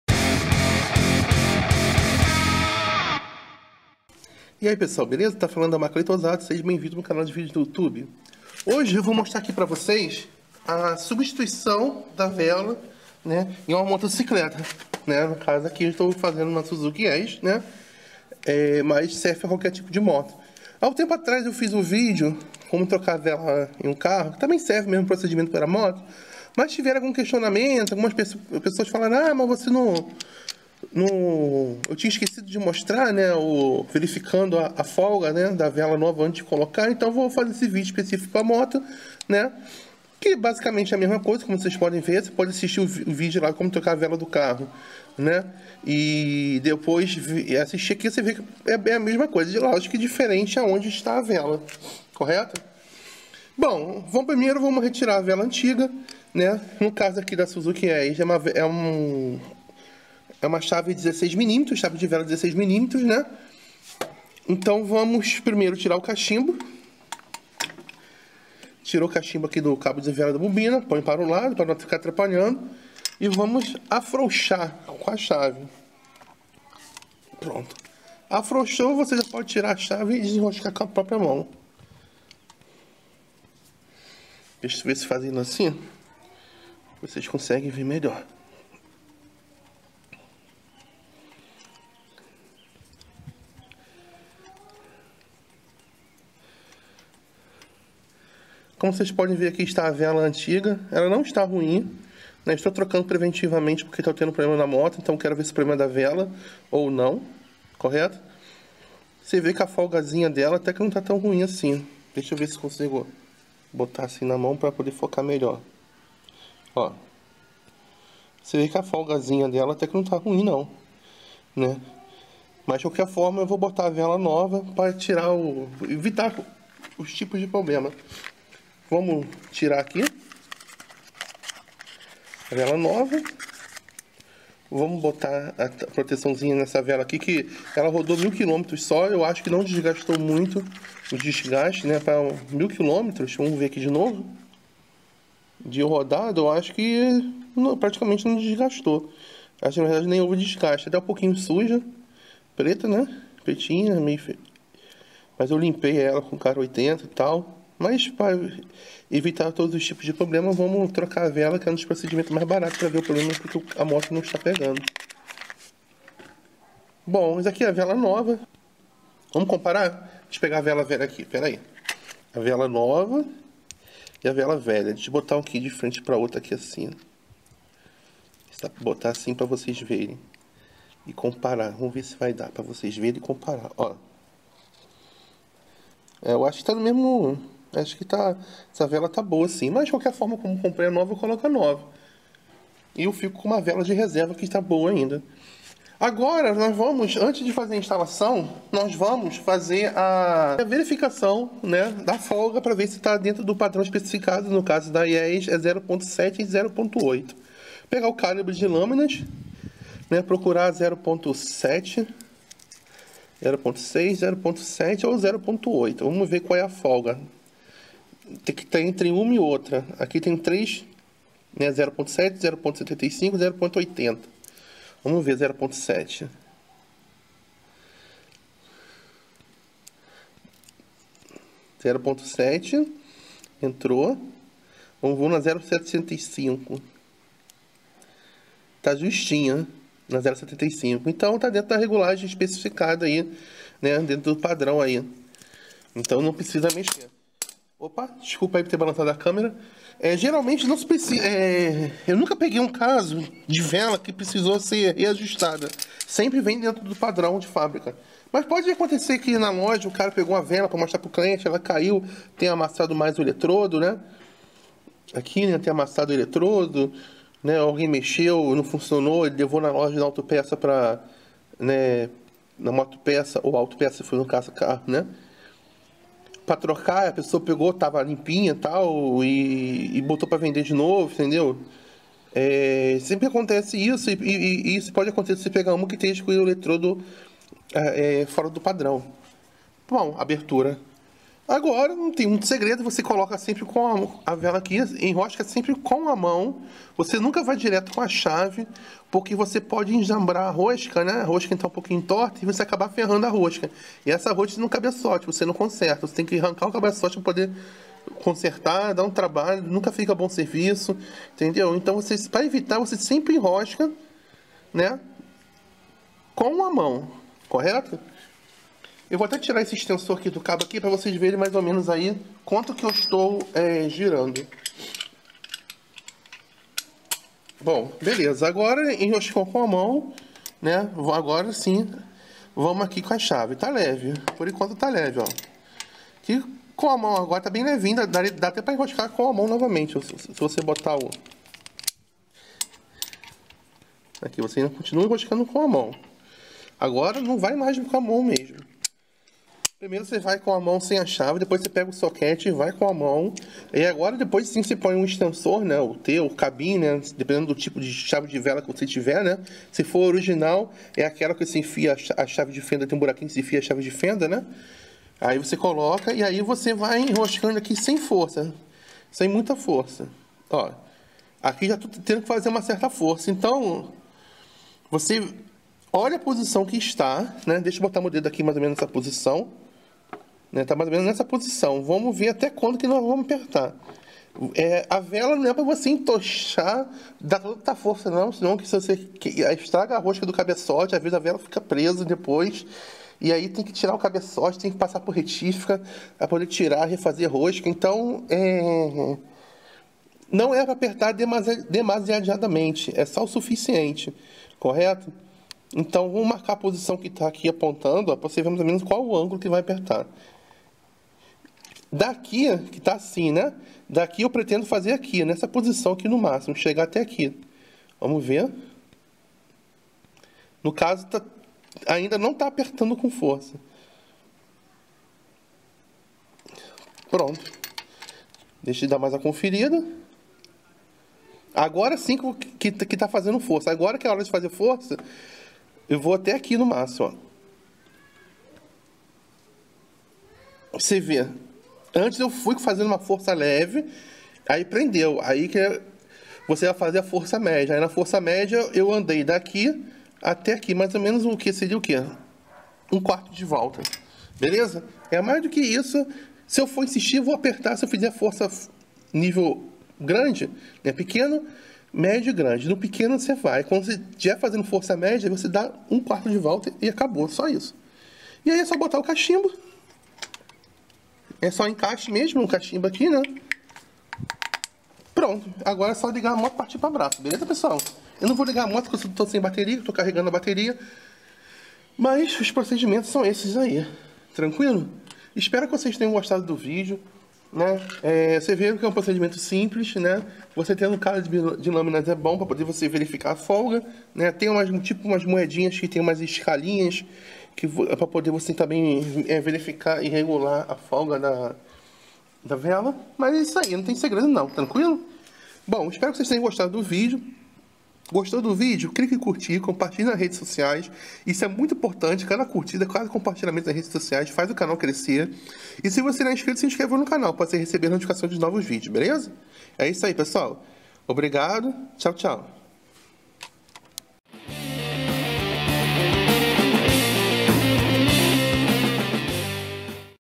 COMO TROCAR VELA IGNIÇÃO DA SUA MOTO
Amigos, desta vez mostro para vocês como fazer a substituição da vela de ignição da sua moto. Apesar do vídeo ter sido realizado em uma Suzuki Yes o mesmo serve de referência para outras motocicletas, como a Suzuki Intruder 125 ou motos da Honda, Yamaha, Dafra ou outra fabricante que utilize um sistema de ignição semelhante.

A um pouco tempo atrás já tinha mostrado como trocar a vela em um carro, procedimento bem parecido, mas ainda restaram algumas dúvidas que procurei sanar neste novo vídeo sendo que desta vez filmado em uma moto.